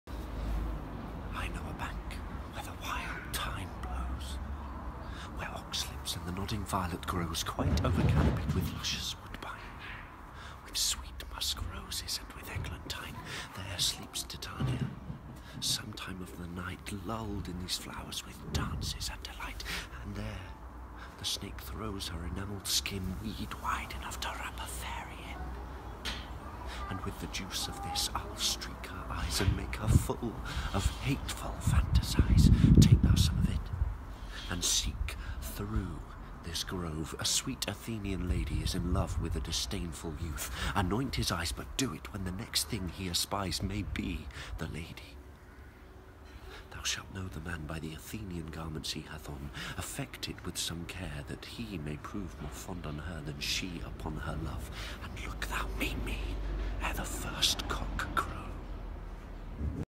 William Shakespeare “I know a bank where the wild thyme blows”
Actor Ben Woods Recites "I know a bank where the wild thyme blows" from Act II, Scene I of Shakespeare's play A Midsummer Nights Dream.

Character - Oberon, King of the Fairies